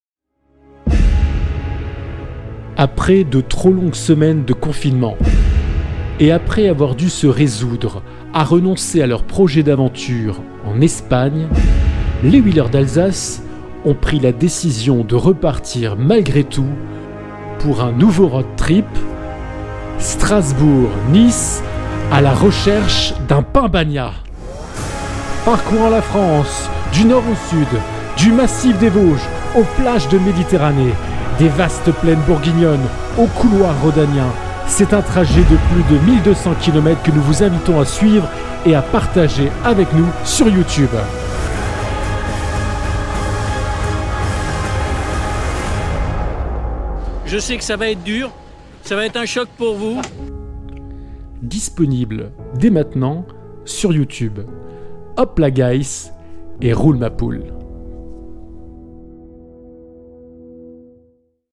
ROADTRIP GYROROUE LES WHEELERS D'ALSACE A LA RECHERCHE DU PAN BAGNAT LE TEASER !!!
Nouvelle aventure pour les wheelers d'Alsace qui vont s'élancer pour un roadtrip de 1200km sur les routes de France pour joindre l'Alsave à la Côte d'Azur !

Un teaser qu'on espère attrayant et donnant envie de partager avec nous un peu de cette aventure !
Après le sel, c'est désormais le Pan Bagnat qui sera notre ultime horizon pour ce périple aux défis nombeux.